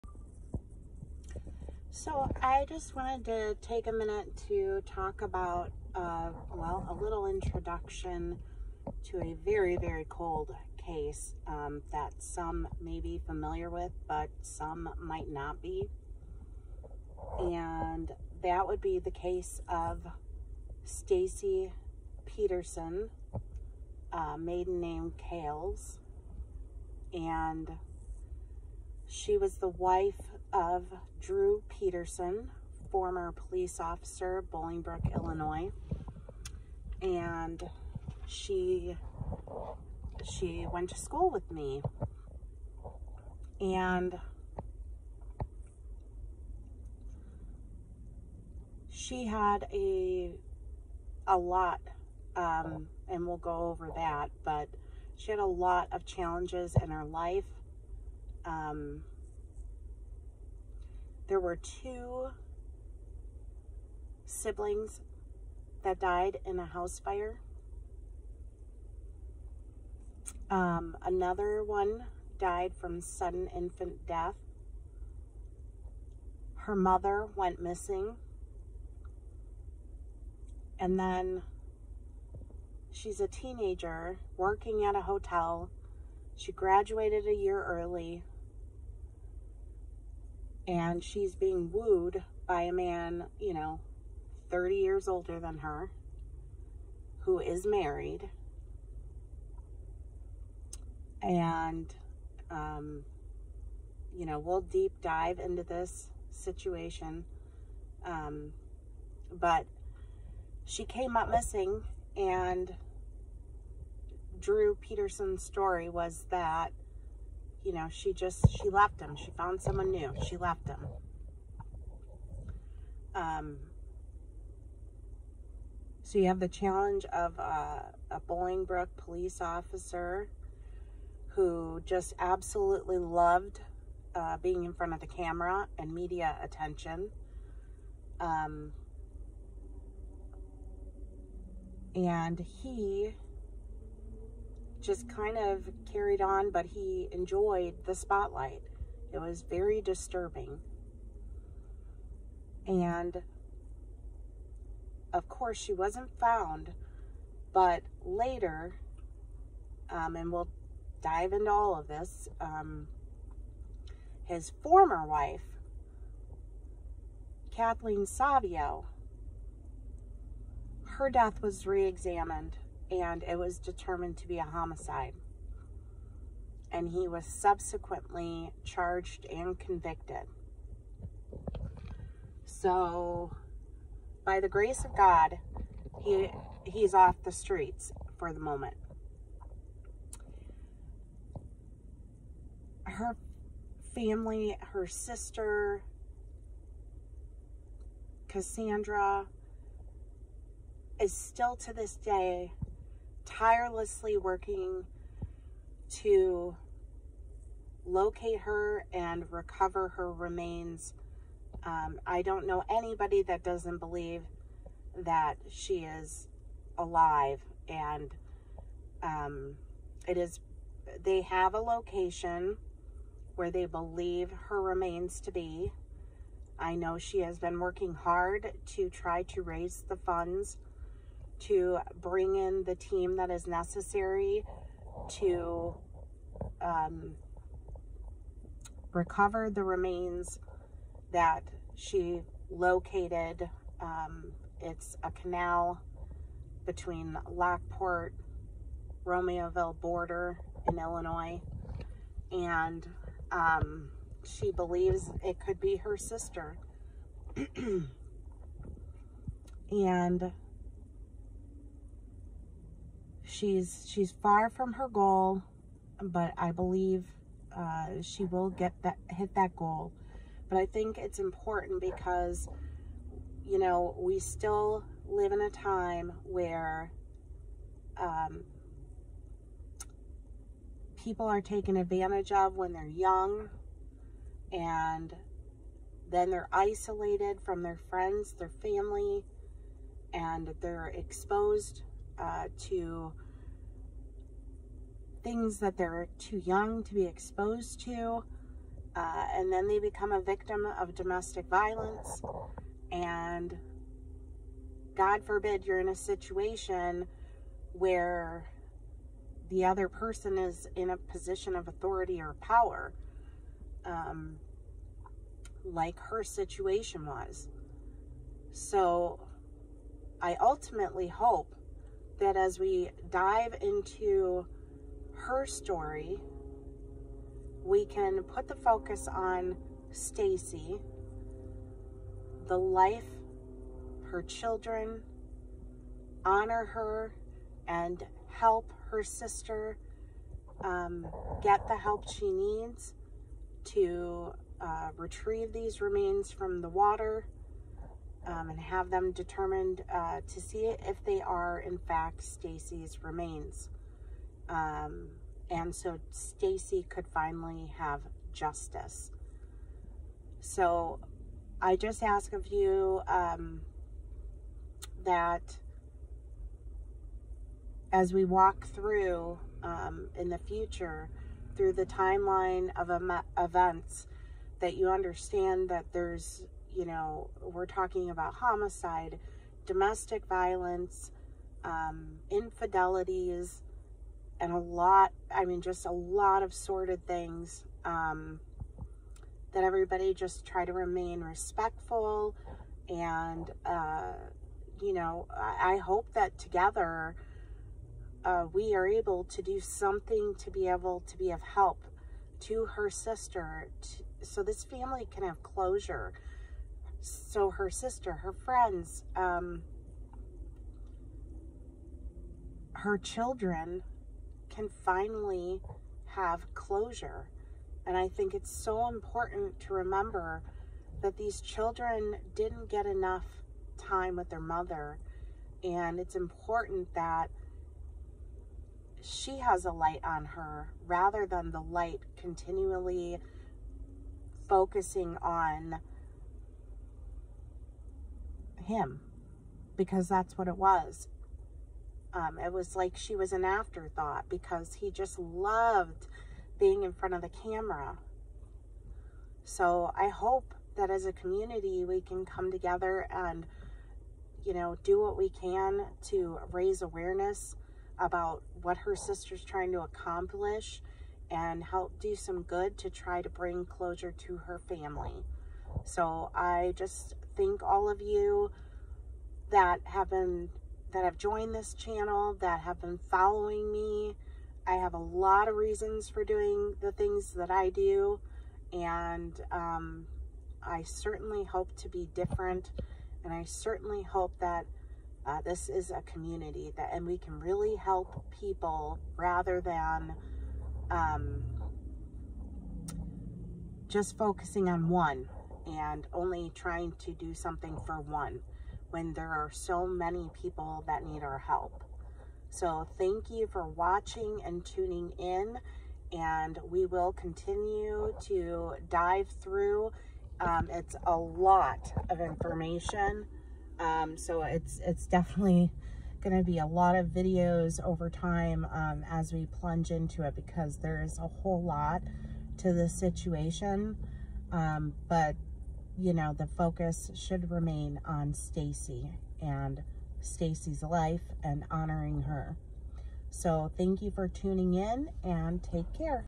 Stacy Cales (Peterson) …The search hasn’t stopped…let’s help her family find her and justice!
Stacy Cales was a beautiful young mother. At the young age of 17 she met her future husband, Bolingbrook IL police officer Drew Peterson. That is when her life would be forever changed…and not for the better! All of these years later her family, friends and children await answers and justice. If you wish to contribute to help her family recover remains they believe to be Stacy, the link for that is